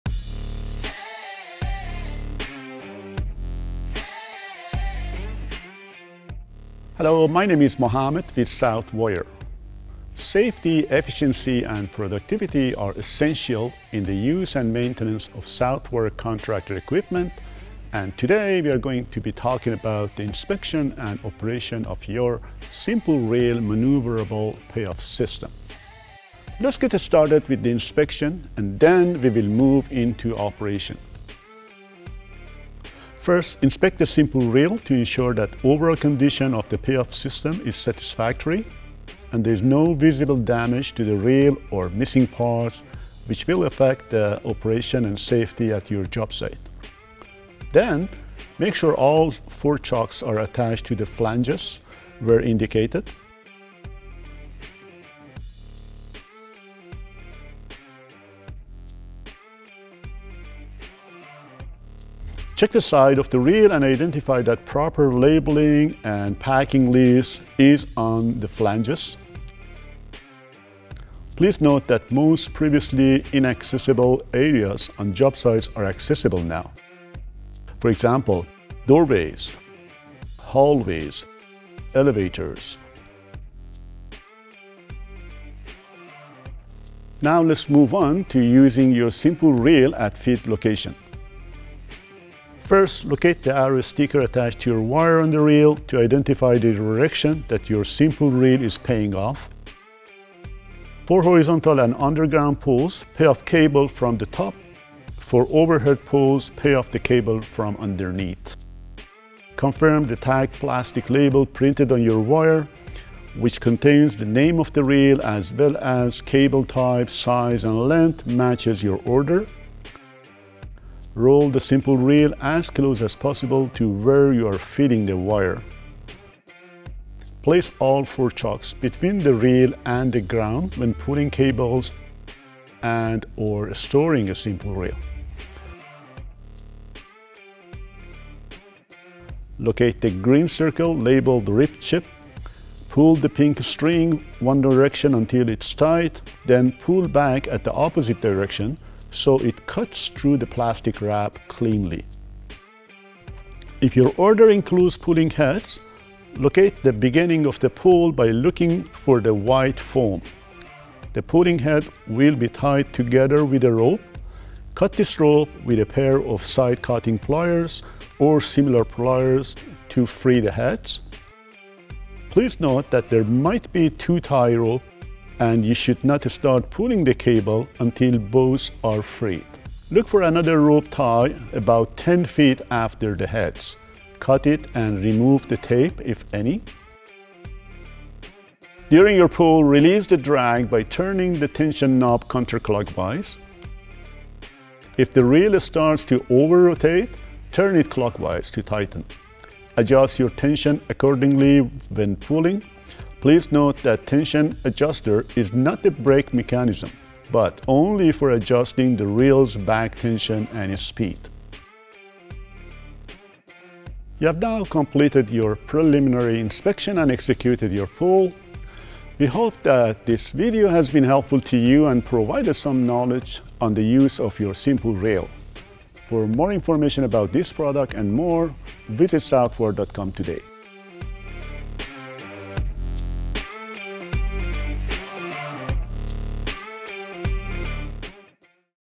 Southwire's SIMpull™ Reel Maneuverable Payoff System Features and Benefits
Southwire's SIMpull™ Reel maneuverable payoff system eliminates the need for additional equipment and makes it possible for one worker to maneuver a 6,000 lb. reel safely and efficiently through narrow halls and doorways.

In this short video, our team showcase the inspection and operation of the SIMpull™ reel maneuverable system